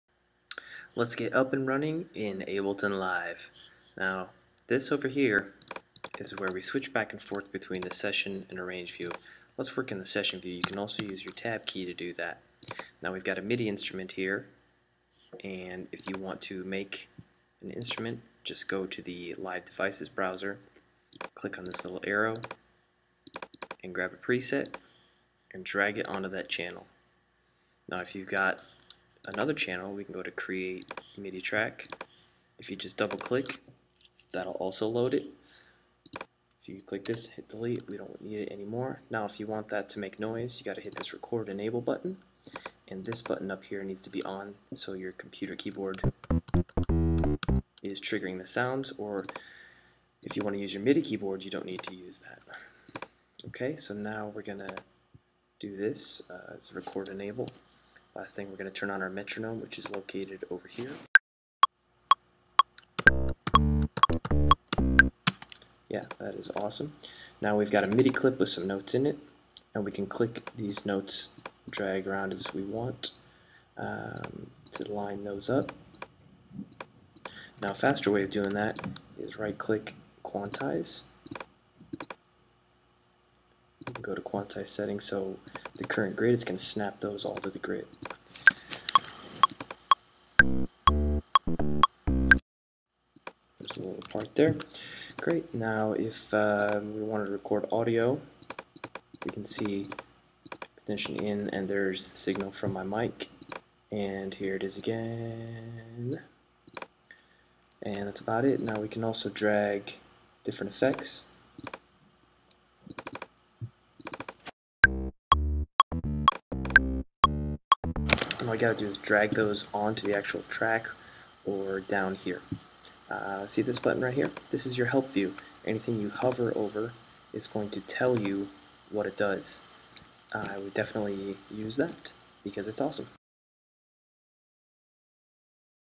Ableton Live Basics Tutorial - Get Started Fast
A blazing walkthrough of Ableton Live 8.  All the basic stuff with none the fluff.  It moves fast so feel free to pause if needed, or ask a question.  I'll get back to you faster if you use twitter.